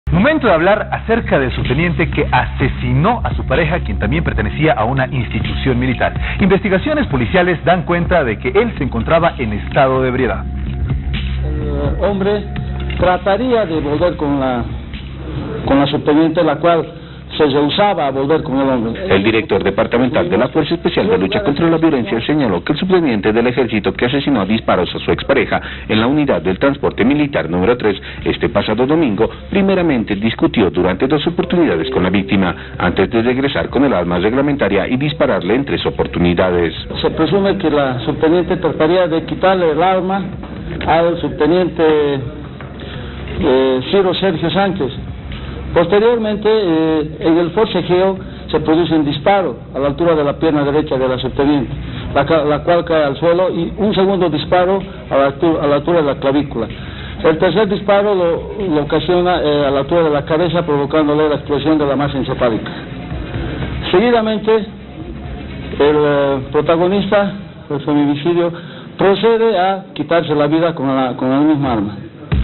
MILITAR MATÓ A SU EX PAREJA Y SE SUICIDÓ
¡Militar asesinó a su ex pareja y se suicidó! En Cochabamba, el uniformado que mantenía una relación amorosa con una subteniente, habría consumido bebidas alcohólicas y tras pelear con la joven, le disparó en la cabeza y luego se quitó la vida con la misma arma. (Video: PAT)